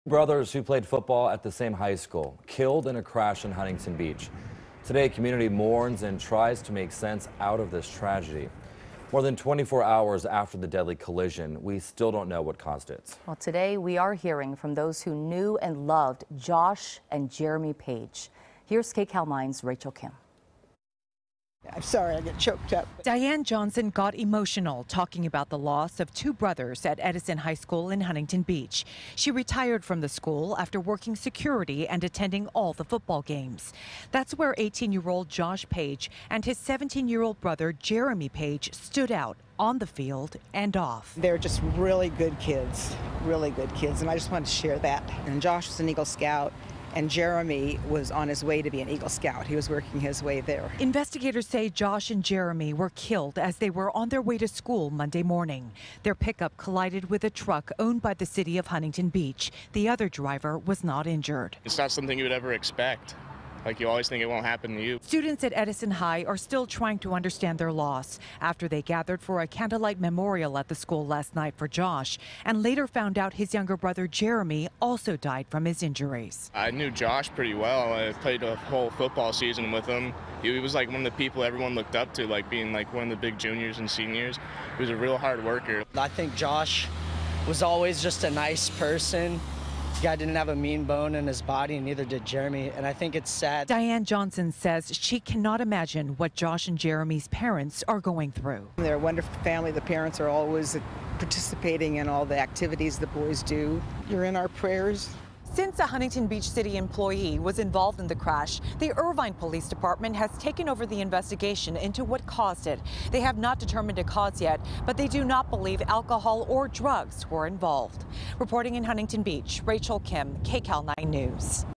Two Brothers Killed In Huntington Beach Crash
Two teenage brothers were killed in a crash in Huntington Beach Monday morning.